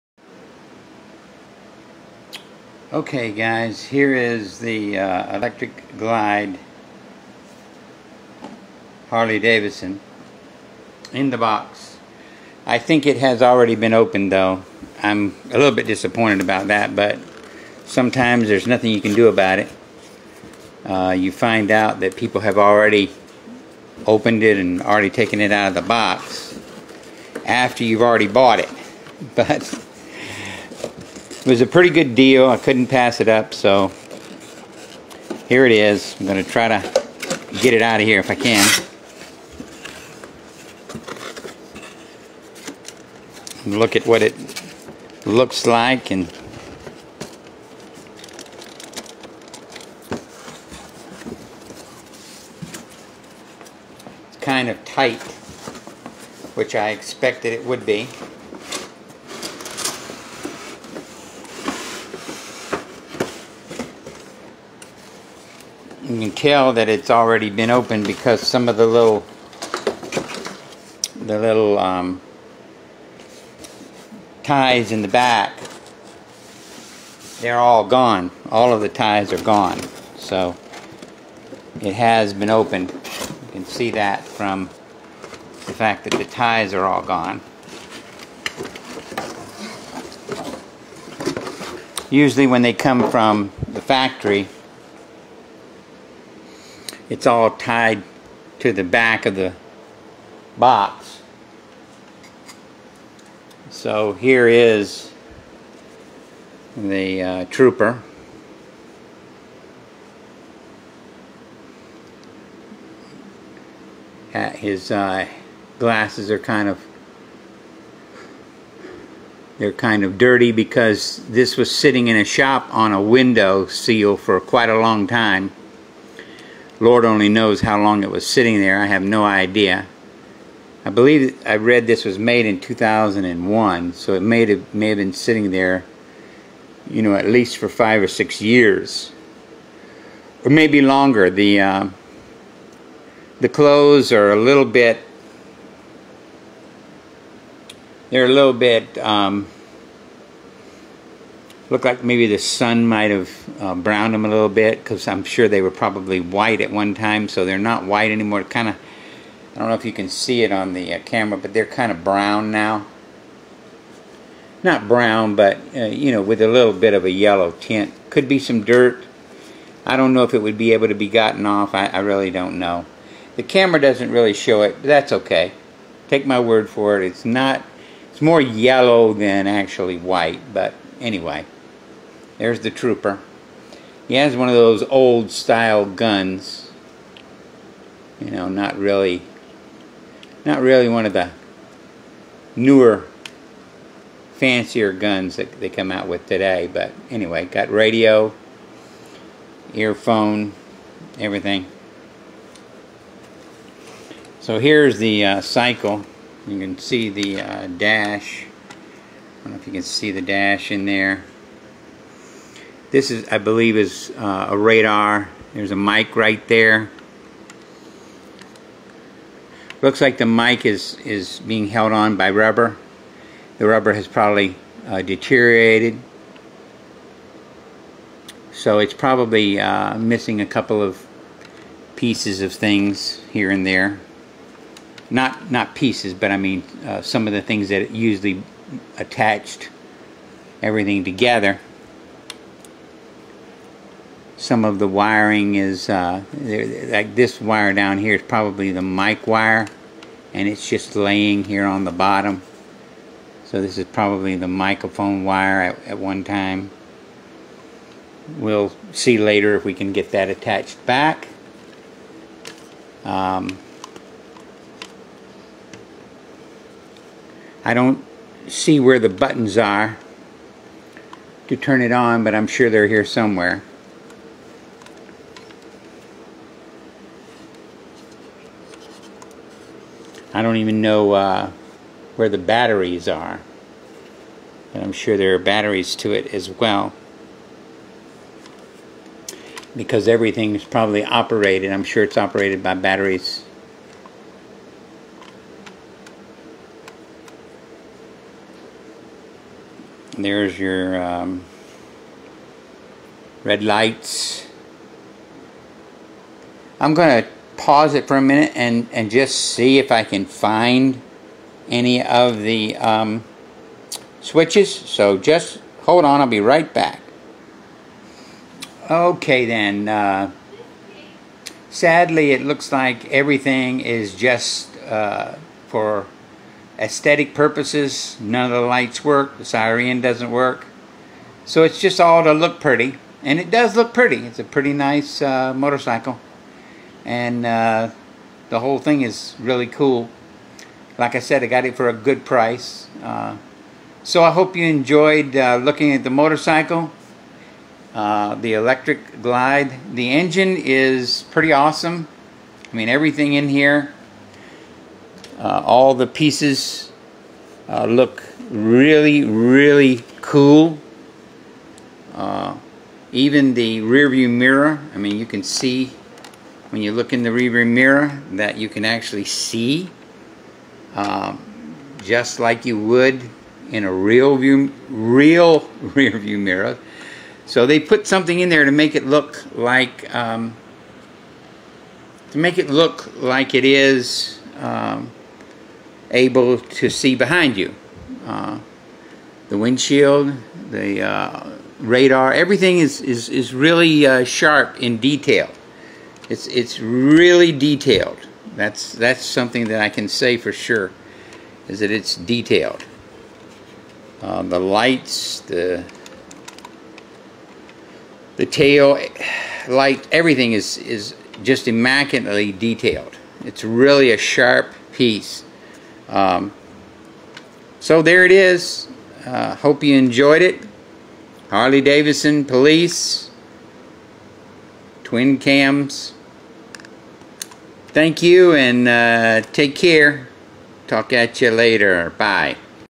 G I Joe Electra Glide Harley-Davidson Motorcycle
Fully Detailed Harley Davidson
12 inch GI Joe Police Officer with Helmet, Sunglasses, tie & boots
Working Stearing Gear from Handlebars to Wheel
Highly Collectable of the GI Joe Series for Harley Cyclists